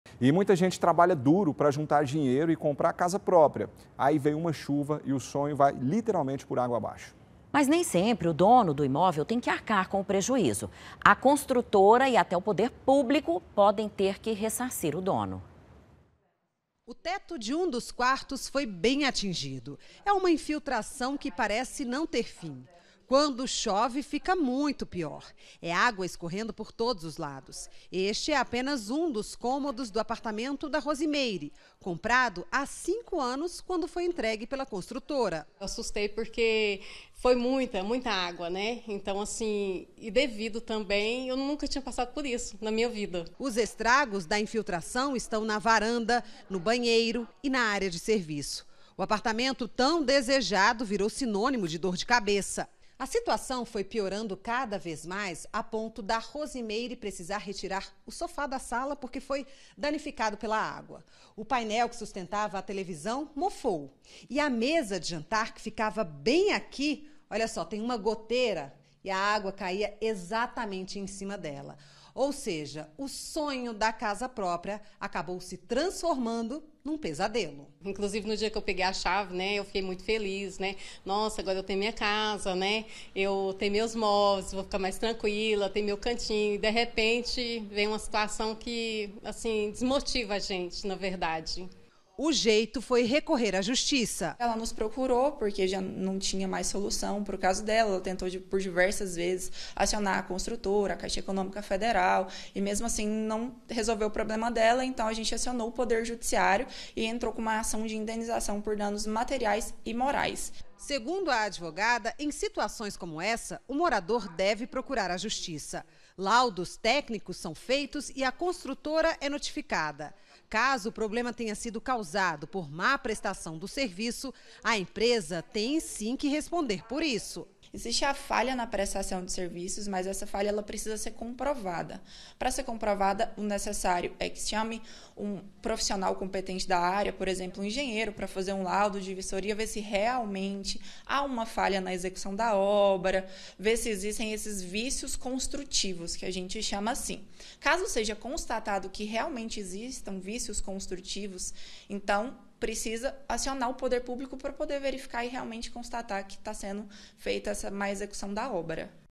PREJUÍZO COM IMÓVEIS: SAIBA COMO ACIONAR A JUSTIÇA PARA COBRAR SOLUÇÃO
MUITA GENTE TRABALHA DURO PRA JUNTAR DINHEIRO E COMPRAR A CASA PRÓPRIA, AÍ VEM UMA CHUVA E O SONHO VAI POR ÁGUA ABAIXO. MAS NEM SEMPRE O DONO DO IMÓVEL TEM QUE ARCAR COM O PREJUÍZO. A CONSTRUTORA E ATÉ O PODER PÚBLICO PODEM TER QUE RESSARCIR O DONO.